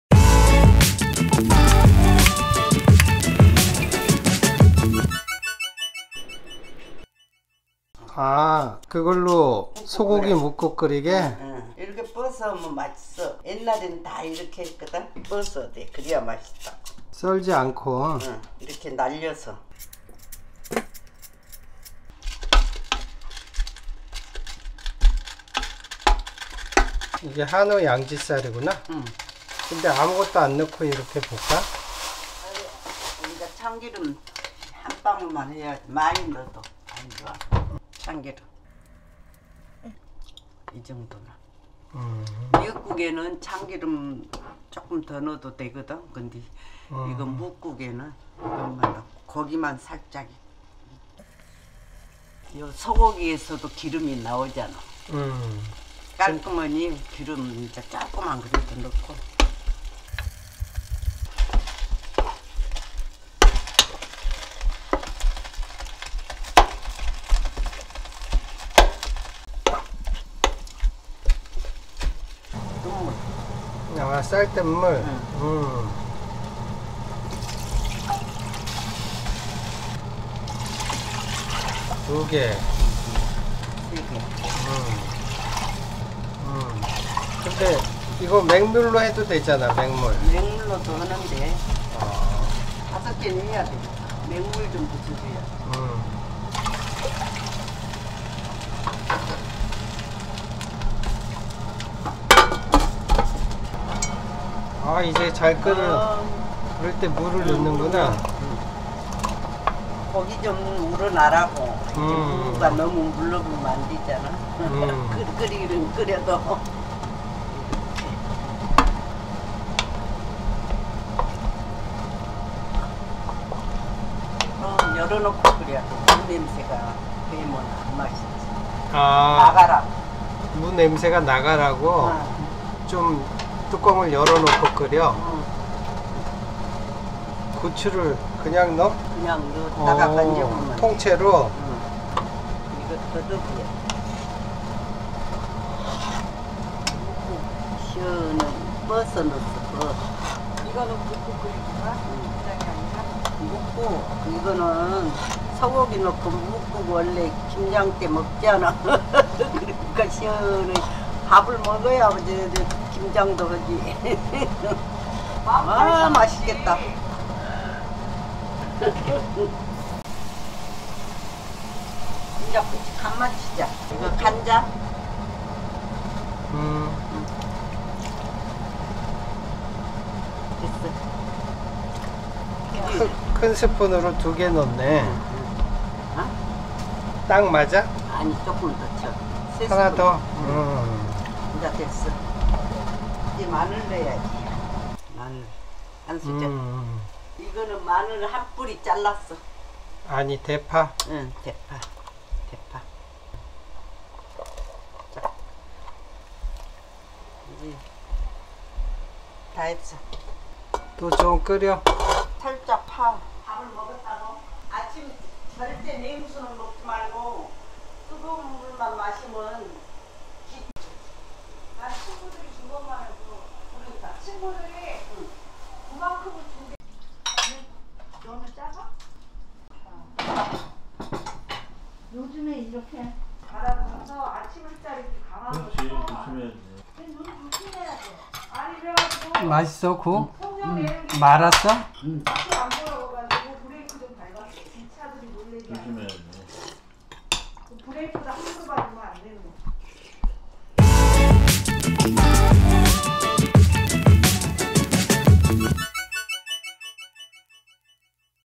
김장날 끓여먹는 소고기무국/양지살 소고기무국
김장하는날 속을 편하고 든든히하려고 끓여봤어요.
한우양지살을 넣고 시원하게끓인 소고기무국 레시피.
[소고기 무국]

*재료
한우양지살 310g
무 1개 510g
대파 1개
참기름 1/2큰술
쌀뜨물 500ccX4  물500CC
청양고추 4개
국간장 2큰술+1큰술
마늘 1스푼 가득

*만드는 법
1.무를 얇게 날려가면 썰어준다.
2.참기름을 조금넣고 양지살을 볶아준다.
3.어느정도 볶다가 쌀뜨물과 물을 넣고 끓여준다.
4.펄펄끓때 무를넣고  끓여준다.
5.고추를 통째로넣고 거품도 제거해준다.
6.국간장과 마늘을 넣어준다.
7.대파를 넣고 잠시끓이면 완성